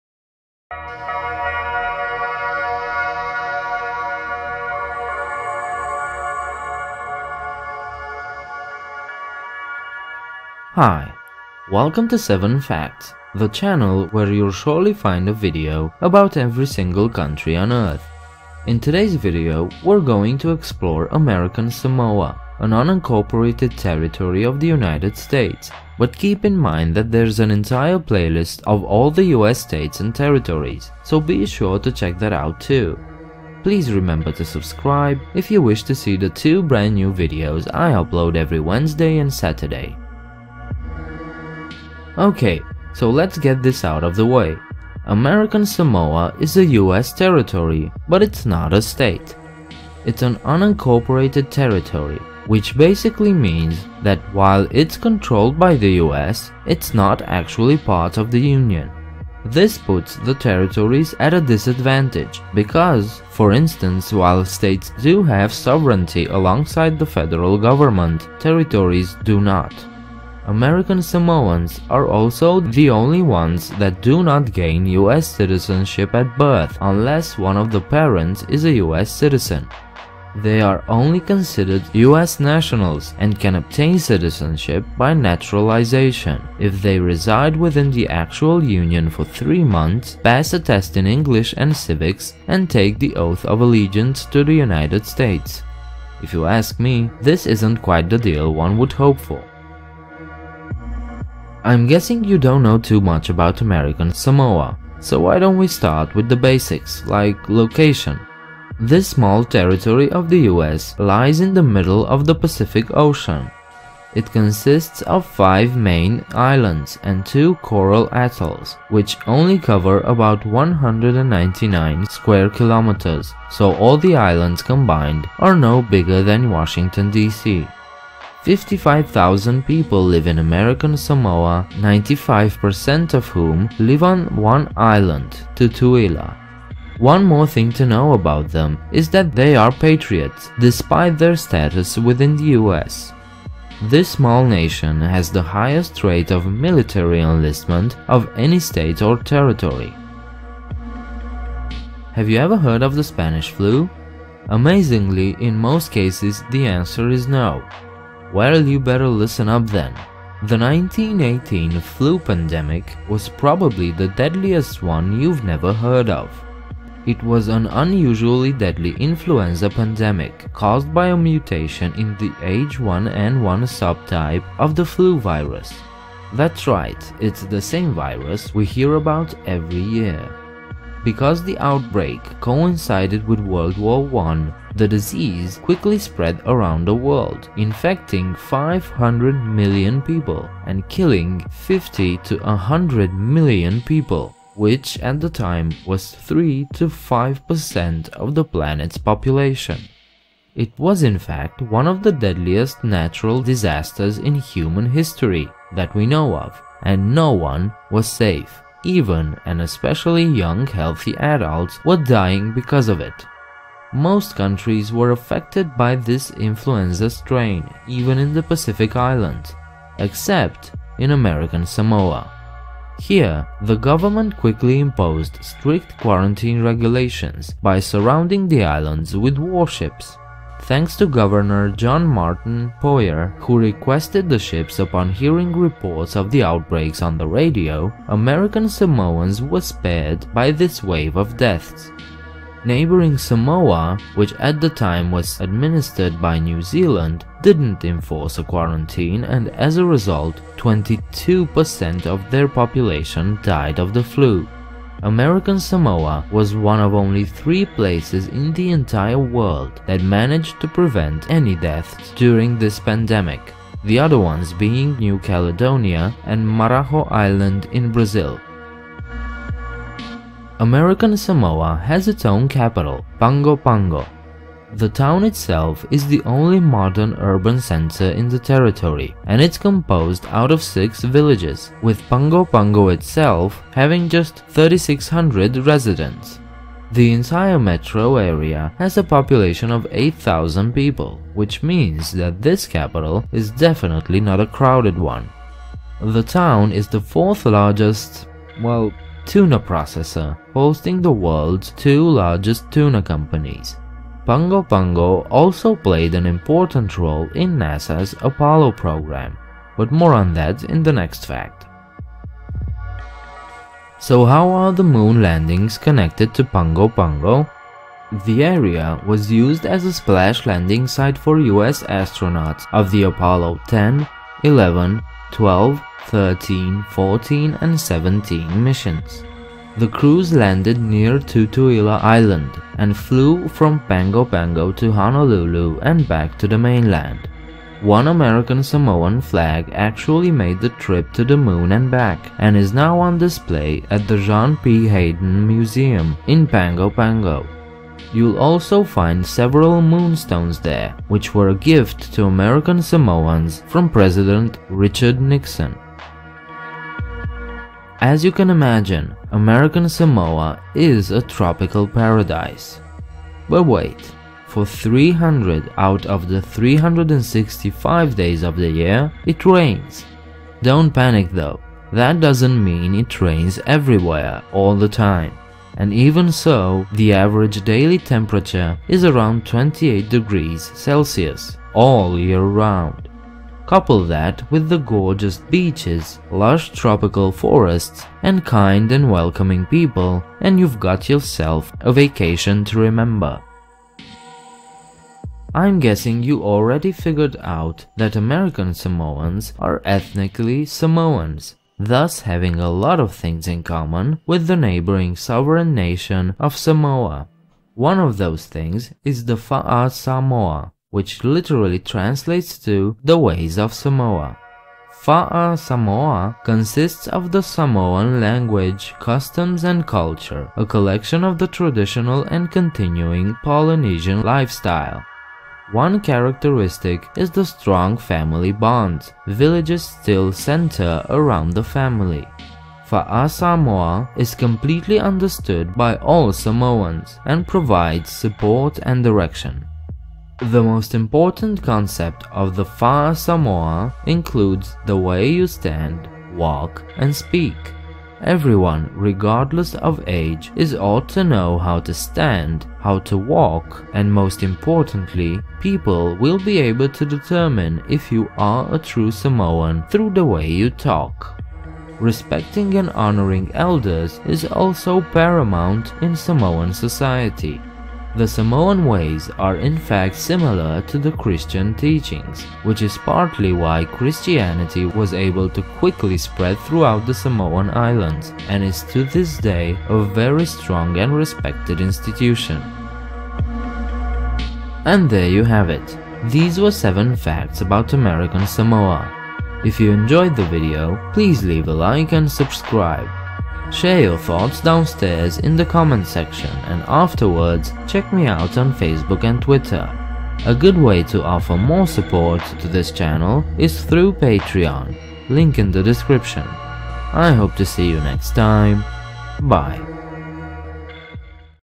7 Facts about American Samoa
In this video you can find seven little known facts about American Samoa.

More information about the video content bellow:
1. American Samoa is classified as an unincorporated, unorganized territory of the United States. Basically, this means American Samoa enjoys diplomatic and military protection from the US, but not all of the provisions and protections of the US Constitution apply. As an unincorporated territory of the United States, American Samoa citizens classify as US Nationals, not US citizens. Most US Nationals consist of citizens of American Samoa.

2. American Samoa consists of five volcanic islands (Tutuila, Aunu'u, and the Manu'a islands of Ofu, Olosega, and Ta'u) and two coral atolls (Rose and Swains atolls).The entire territory of American Samoa consists of 117,500 square miles (30,400 square km), which is about the size of Oregon or New Zealand. However, only 76.1 square miles (197 square km), or 0.1% of the territory, is actually dry land.

3. During the deadly worldwide outbreak of influenza in 1918, the United States enforced strict quarantine regulations in American Samoa, protecting the islands and the people from the disease. New Zealand, however, who at the time held control of the Independent State of Samoa, did not enforce a quarantine, resulting in the death of 8,500, or 22% of the population in the western Samoan islands. American Samoa became one of only three places in the world (the others being New Caledonia and Marajó island in Brazil) to have prevented any deaths during the pandemic through the quick response from Governor John Martin Poyer after hearing news reports of the outbreak on the radio and requesting quarantine ships from the U.S. mainland.

4. American Samoa has a population of 54,343 for the year 2015. Tutuila island has 48,000 people alone, making up about 88% of the total population. Pago Pago is the capital of American Samoa.

5. American Samoa's waters were used as a splash landing for US astronauts during the moon landings in the 60s and 70s. A small American Samoa flag was sent to the moon on one of the missions, and later presented along with three moon rocks to American Samoa by US President Richard Nixon.

6. Weather stations detect rain 300 days out of 365 each year in American Samoa.

7. Fa'asamoa, or the Samoa way of life, still dominates the way American Samoans live. Villages center around the family, and the Matai, family chief, dictates the community activities. In addition, 90% of American Samoa land is communally owned by the Samoan families.

Images:
By TUBS - Own work. This vector graphics image was created with Adobe Illustrator. This file was uploaded with Commonist.

Intro Creator:
Pushed to Insanity